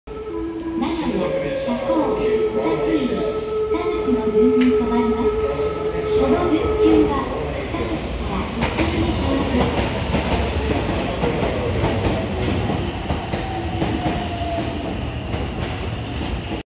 ●20200909 3300系 到着 ＠金山
撮影路線：名鉄 名古屋本線
撮影駅：金山
行先：準急 弥富
車両：3300系
両数：4両
撮影機材：【スマホ】SONY Xperia XZ2 Premium
MH：ーー
備考欄：